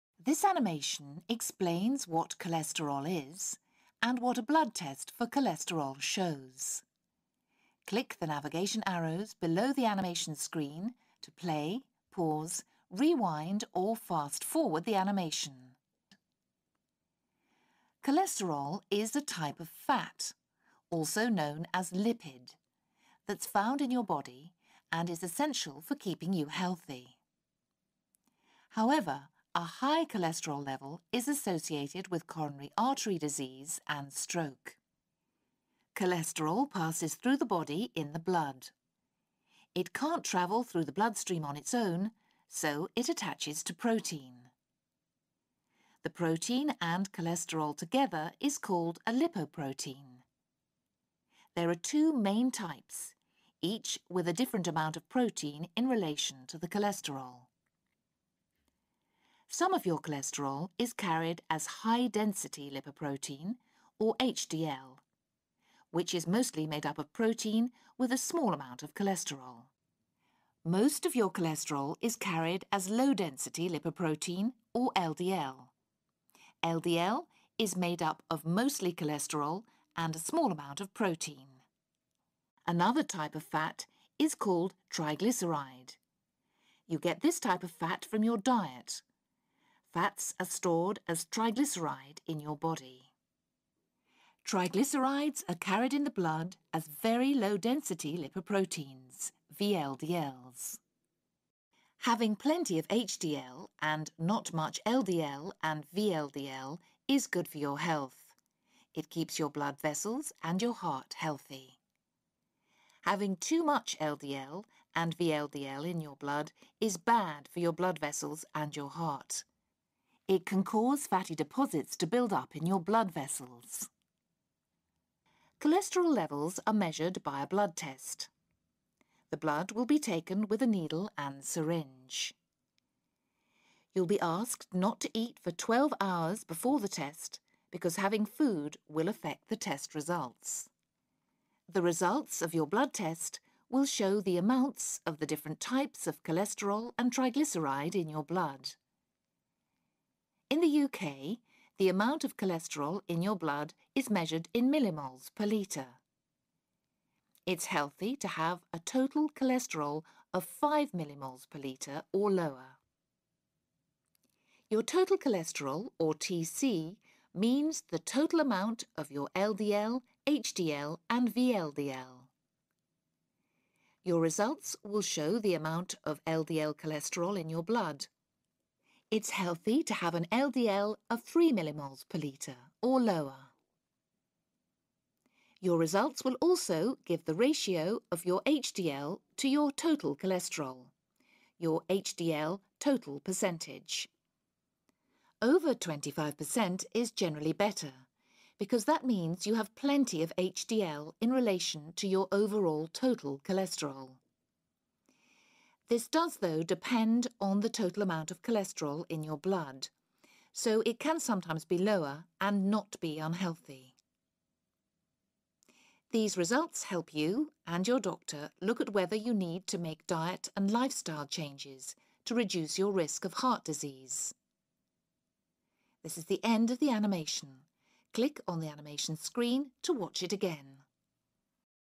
Colesterol
Qué es el colesterol y qué muestran los análisis de sangre para el colesterol alto